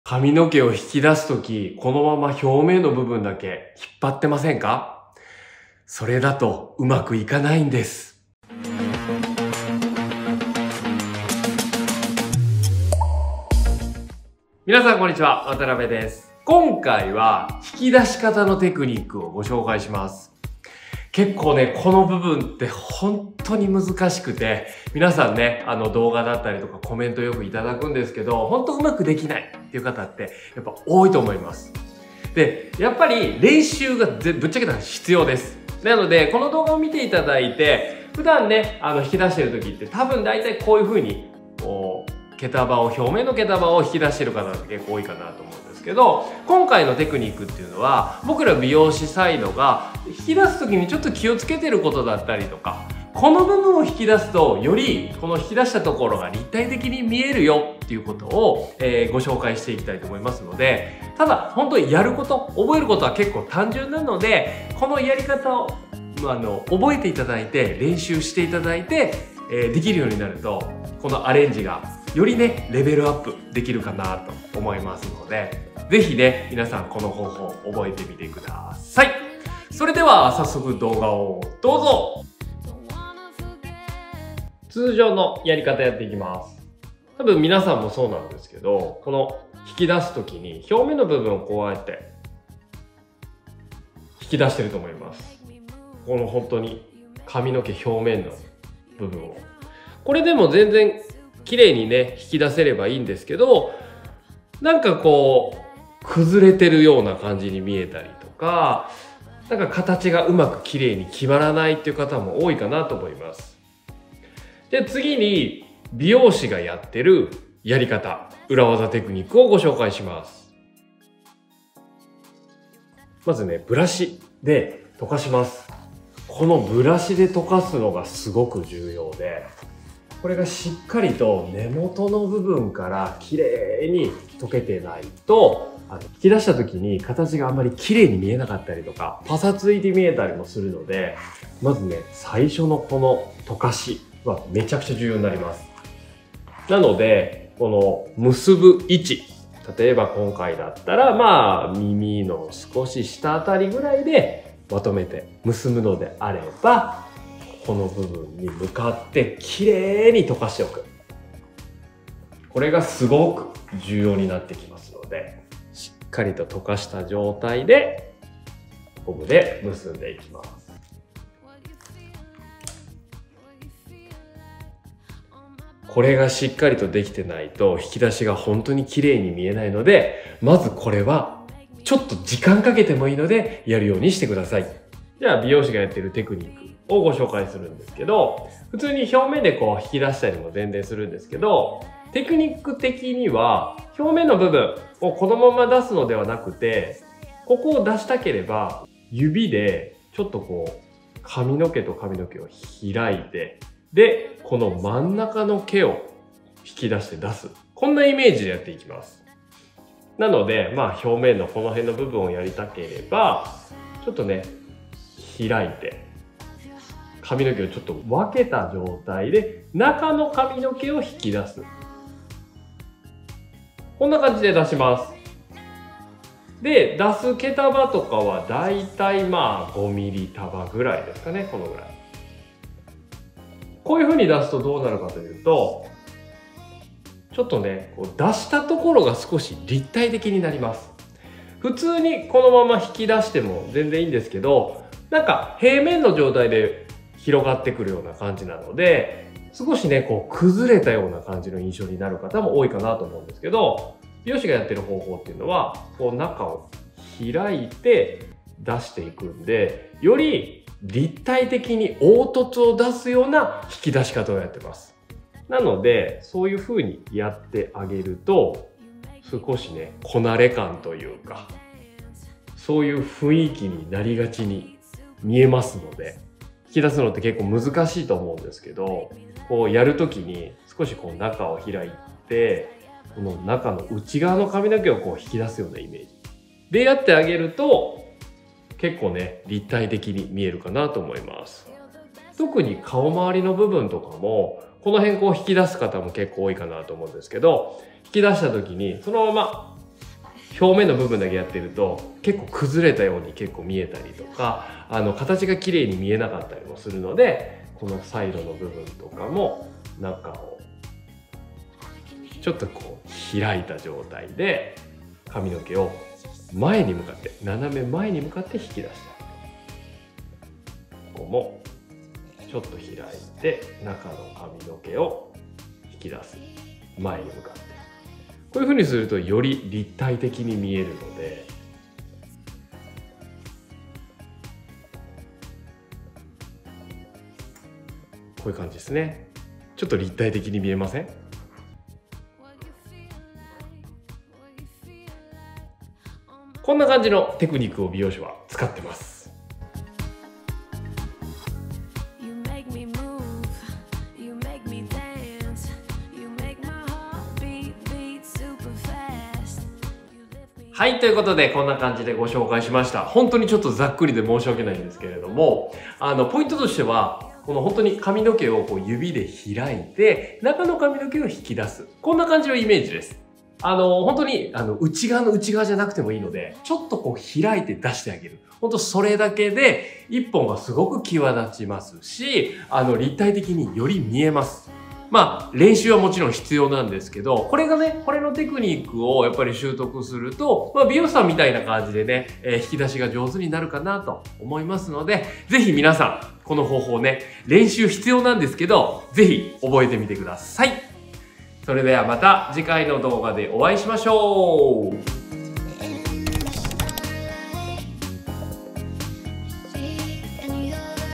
ヘアアレンジが上手くなる！！美容師がやっている引き出しテクニック。表参道美容師 SALONTube 渡邊義明
こんにちは

今回は皆さんからコメントを多く頂くことが多い
引き出しのテクニックをご紹介です

通常のやり方と美容師のやり方を両方やっていますので
ぜひ参考にしていただけたらと思います😊

※ただ見ただけでは出来ません。
あくまでも練習は必要ですよ！！

Hair styling
Hair arrangement
头发
헤어